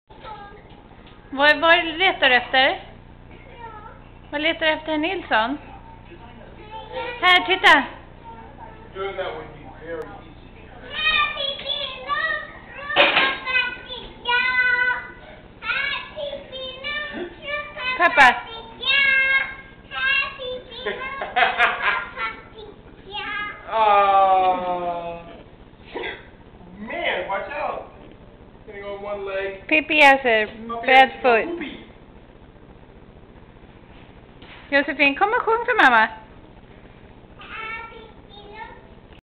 2008 01 josefine as Pippi longstocking
josefine as Pippi longstocking, after surgery on her foot